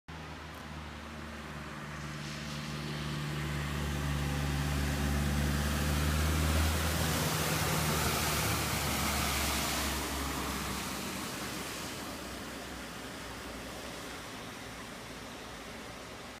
Inondations à Mondorf-les-Bains, 30 mai 2016
Suite aux intempéries, certaines villes se sont retrouvées sous les eaux, comme ici à Mondorf-les-Bains. Images: Catherine Thomas